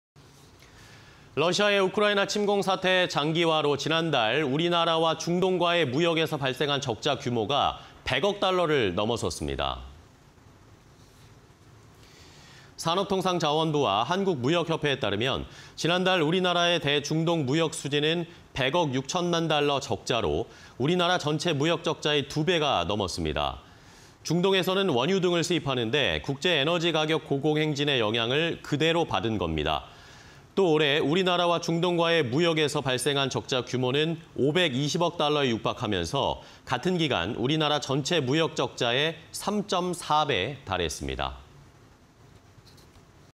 7월 중동 무역적자 100억달러 돌파... 에너지 가격 급등 탓 (20220803)
러시아의 우크라이나 침공 사태 장기화로 지난달 우리나라와 중동과의 무역에서 발생한 적자 규모가 100억달러 선을 처음으로 넘어선 것으로 나타났습니다.
산업통상자원부와 한국무역협회에 따르면 지난달 우리나라의 대 중동 무역수지는 100억6천만달러 적자로 우리나라 전체 무역적자의 2배가 넘었습니다.
중동에서는 원유 등을 수입하는데 국제 에너지 가격 고공행진의 영향을 그대로 받은 겁니다.
또 올해 우리나라와 중동과의 무역에서 발생한 적자 규모는 520억달러에 육박하면서 같은 기간 우리나라 전체 무역적자(-150억2000만달러)의 3.4배에 달했습니다.